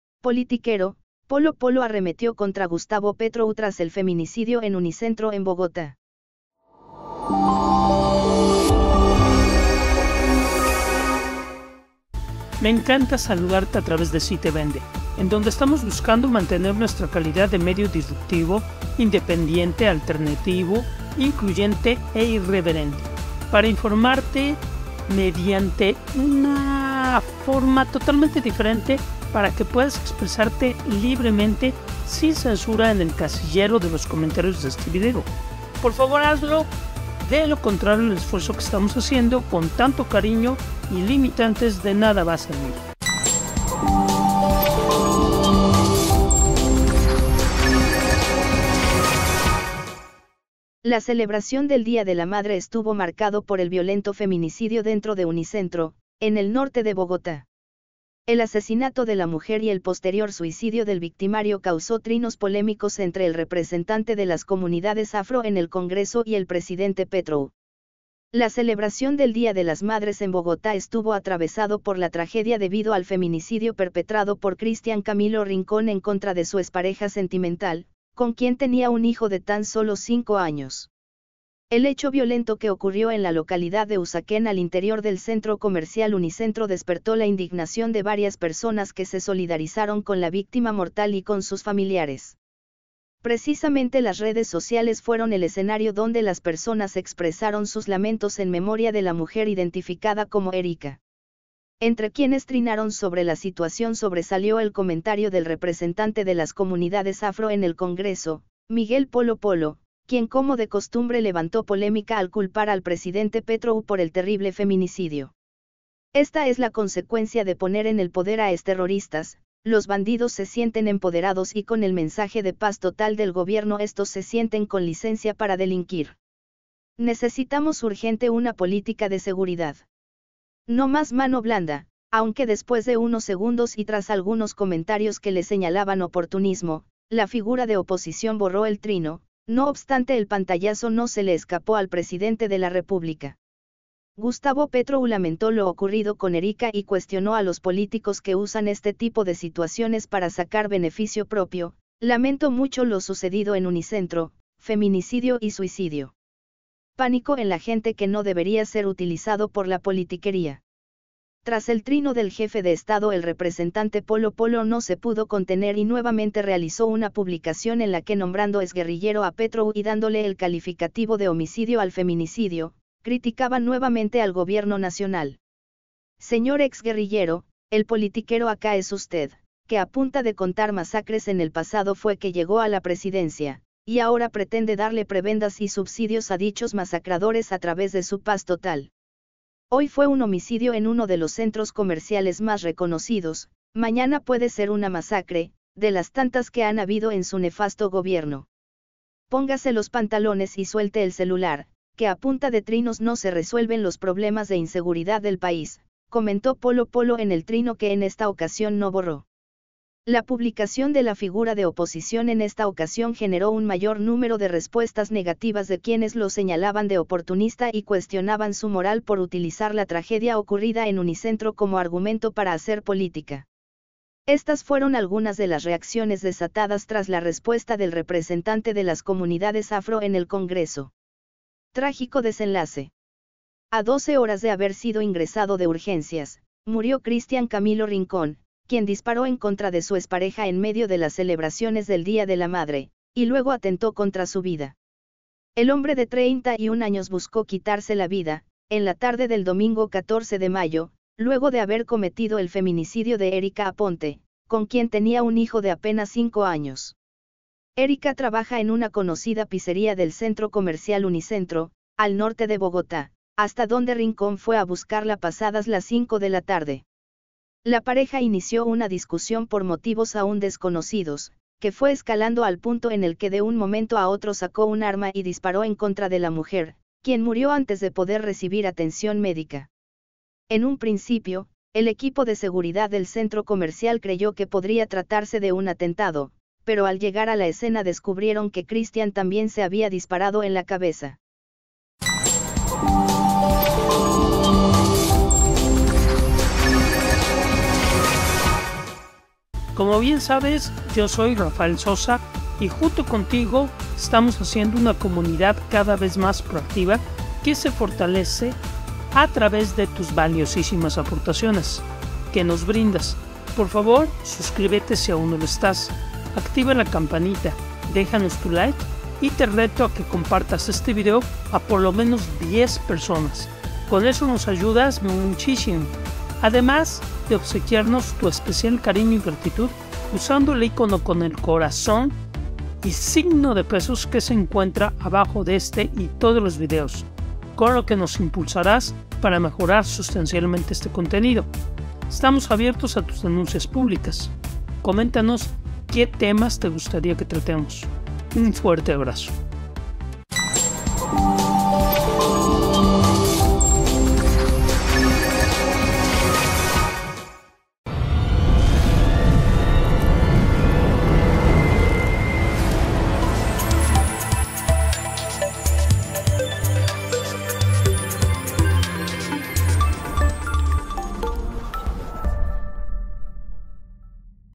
‘Politiquero’: Polo Polo arremetió contra Gustavo Petro tras el feminicidio en Unicentro en Bogotá
• ‘Politiquero’: Polo Polo arremetió contra Gustavo Petro tras el feminicidio en Unicentro en Bogotá. La celebración del Día de la Madre estuvo marcado por el violento feminicidio dentro de Unicentro, en el norte de Bogotá. El asesinato de la mujer y el posterior suicidio del victimario causó trinos polémicos entre el representante de las comunidades afro en el Congreso y el presidente Petro. La celebración del Día de las Madres en Bogotá estuvo atravesado por la tragedia debido al feminicidio perpetrado por Christian Camilo Rincón en contra de su expareja sentimental, con quien tenía un hijo de tan solo cinco años. El hecho violento que ocurrió en la localidad de Usaquén al interior del Centro Comercial Unicentro despertó la indignación de varias personas que se solidarizaron con la víctima mortal y con sus familiares.. Precisamente las redes sociales fueron el escenario donde las personas expresaron sus lamentos en memoria de la mujer identificada como Erikha. Entre quienes trinaron sobre la situación sobresalió el comentario del representante de las comunidades afro en el Congreso, Miguel Polo Polo, quien como de costumbre levantó polémica al culpar al presidente Petro por el terrible feminicidio.. “Esta es la consecuencia de poner en el poder a exterroristas; los bandidos se sienten empoderados y con el mensaje de paz total del Gobierno estos se sienten con licencia para delinquir. ¡Necesitamos urgente una política de seguridad! No más mano blanda”, aunque después de unos segundos y tras algunos comentarios que le señalaban oportunismo, la figura de oposición borró el trino, no obstante el pantallazo no se le escapó al presidente de la República.. Gustavo Petro lamentó lo ocurrido con Erikha y cuestionó a los políticos que usan este tipo de situaciones para sacar beneficio propio: “Lamento mucho lo sucedido en Unicentro: Feminicidio y suicidio. Pánico en la gente que no debería ser utilizado por la politiquería”.. Tras el trino del jefe de Estado el representante Polo Polo no se pudo contener y nuevamente realizó una publicación en la que nombrando exguerrillero a Petro y dándole el calificativo de homicidio al feminicidio, criticaba nuevamente al Gobierno nacional.. “Señor ex guerrillero: el politiquero acá es usted, que a punta de contar masacres en el pasado fue que llegó a la presidencia, y ahora pretende darle prebendas y subsidios a dichos masacradores a través de su “paz total”. Hoy fue un homicidio en uno de los centros comerciales más reconocidos, mañana puede ser una masacre, de las tantas que han habido en su nefasto gobierno. Póngase los pantalones y suelte el celular, que a punta de trinos no se resuelven los problemas de inseguridad del país”, comentó Polo Polo en el trino que en esta ocasión no borró.. La publicación de la figura de oposición en esta ocasión generó un mayor número de respuestas negativas de quienes lo señalaban de oportunista y cuestionaban su moral por utilizar la tragedia ocurrida en Unicentro como argumento para hacer política.. Estas fueron algunas de las reacciones desatadas tras la respuesta del representante de las comunidades afro en el Congreso:. A 12 horas de haber sido ingresado de urgencias, murió Christian Camilo Rincón, quien disparó en contra de su expareja en medio de las celebraciones del Día de la madre, y luego atentó contra su vida.. El hombre de 31 años buscó quitarse la vida, en la tarde del domingo 14 de mayo, luego de haber cometido el feminicidio de Érika Aponte, con quien tenía un hijo de apenas cinco años.. Érika trabaja en una conocida pizzería del Centro Comercial Unicentro, al norte de Bogotá, hasta donde Rincón fue a buscarla pasadas las 5 de la tarde.. La pareja inició una discusión por motivos aún desconocidos, que fue escalando al punto en el que de un momento a otro sacó un arma y disparó en contra de la mujer, quien murió antes de poder recibir atención médica.. En un principio, el equipo de seguridad del Centro Comercial creyó que podría tratarse de un atentado, pero al llegar a la escena descubrieron que Christian también se había disparado en la cabeza.